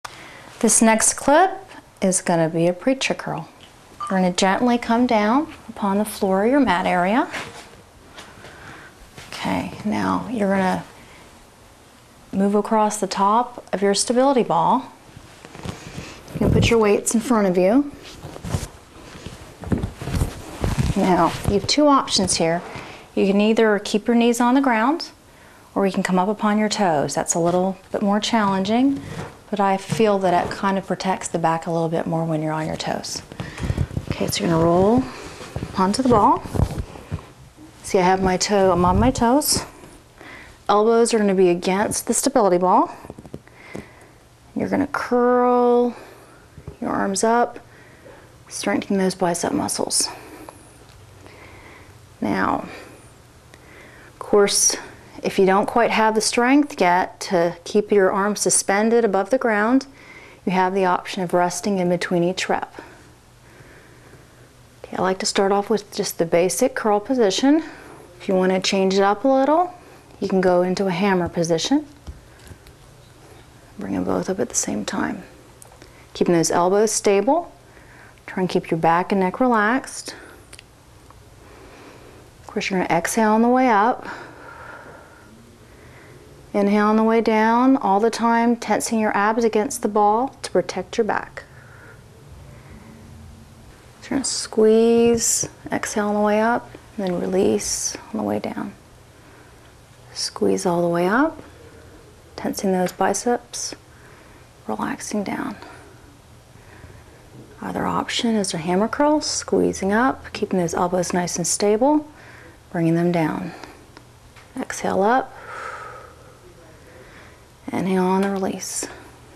Stability Ball Workout : Stability Ball: Preacher Curl
You can use either your knees or toes to steady yourself as you do preacher curls with your stability ball. Learn how to do a full body workout with a stability ball in this free fitness video.

Bio: Sue Conrad is a personal trainer with a certification through the American Council on Exercise (ACE) since 2001. She currently teaches cardio kickboxing and trains clients of various ages.
Filmmaker: Patrick Eaves